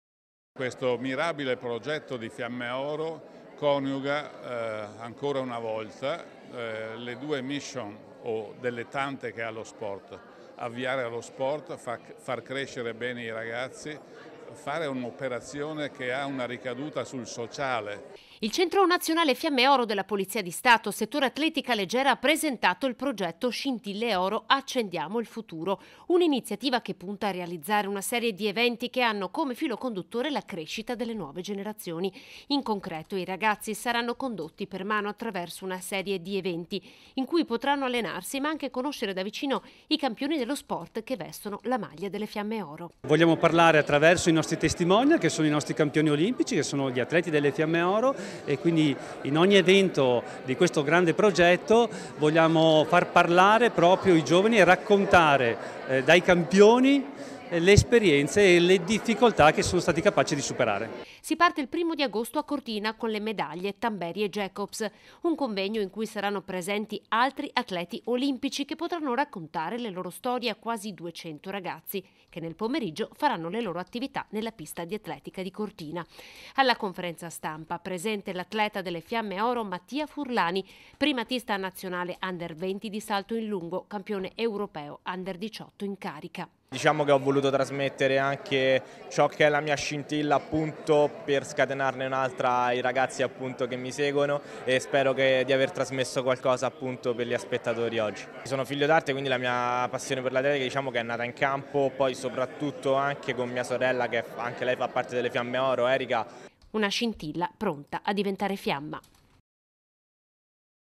ATLETICA LEGGERA: DARE VOCE ALLE 'SCINTILLE' AFFINCHE' DIVENTINO 'FIAMME' | 29/07/2023
TG Padova Sera 29/07/2023 - PADOVA - Il Centro Nazionale Fiamme Oro della Polizia di Stato settore Atletica Leggera ha presentato il progetto “SCINTILLE ORO – Accendiamo il futuro”: una serie di eventi dedicati ai più giovani e alle loro "scintille" affinchè possano diventare fiamme || Il Centro Nazionale Fiamme Oro della Polizia di Stato - settore Atletica Leggera ha presentato il progetto “SCINTILLE ORO – Accendiamo il futuro”, un’iniziativa che punta a realizzare una serie di eventi che ha come filo conduttore la crescita delle nuove generazioni. in concreto i ragazzi saranno condotto per mano attraverso una serie di eventi in cui potranno allenarsi ma anche conoscere da vicino campioni dello sport che vestono la maglia delle Fiamme Oro.Si parte il primo di agosto a Cortina con le medaglie Tamberi e Jacobs, un convengo in cui saranno presenti altri atleti olimpici che potranno raccontare le loro storie a quasi 200 ragazzi che nel pomeriggio faranno le loro attività nella pista di atletica di Cortina. Alla conferenza stampa presente l’atleta delle Fiamme Oro Mattia Furlani, primatista nazionale under 20 di salto in lungo, campione europeo under 18 in carica. - Intervistati DINO PONCHIO (PRESIDENTE CONI VENETO), SERGIO BALDO (COORD. CENTRO REGIONALE FIAMME ORO ATLETICA LEGGERA), MATTIA FURLANI (ATLETA FIAMME ORO) (Servizio di Anna De Roberto) - Segui Telenordest anche sul digitale terrestre! Visita il sito